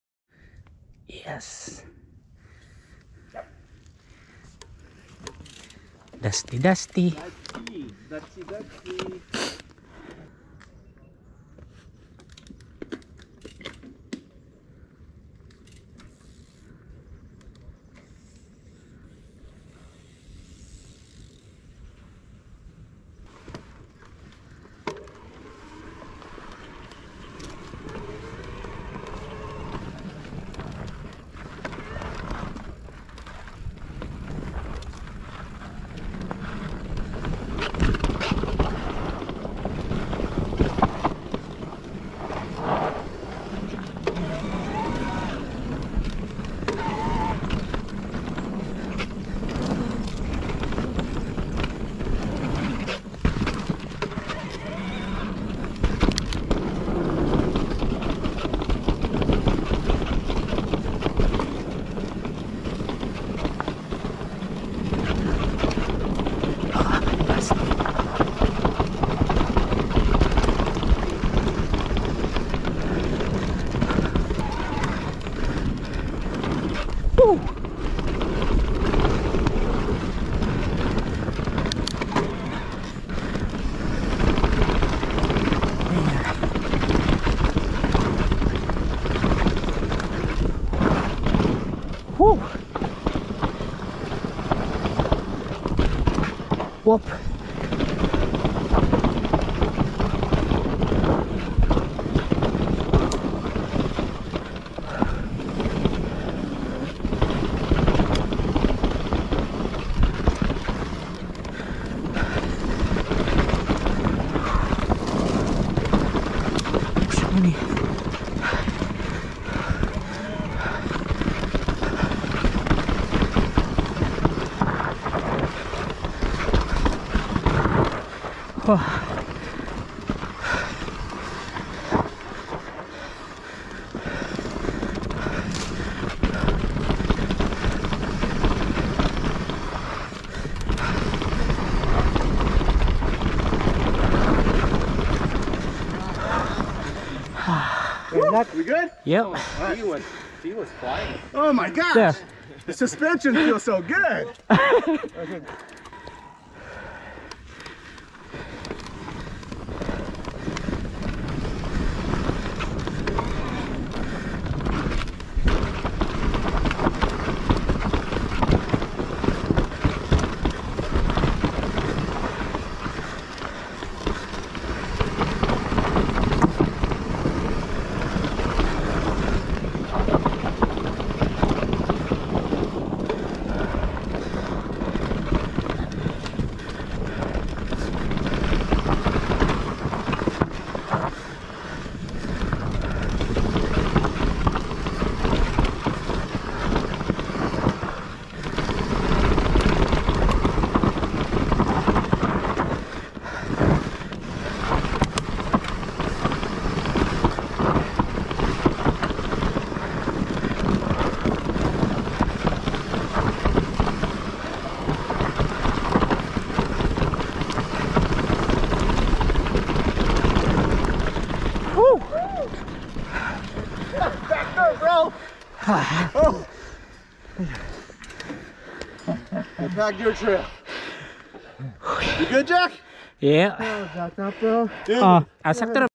Riding 10psi with Tannus Armour MTB insert of my Marin Alpine Trail E1
I did not know both tires was 10psi until I checked after this ride. Thankful for the Tannus Armour insert not ruining the wheels. Great riding with my idols...